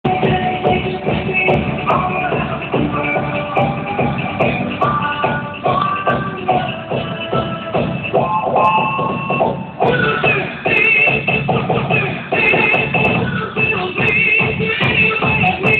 abdominales
ya para terminar la sesion de ejercicios diarios, cerramos con las abdominales.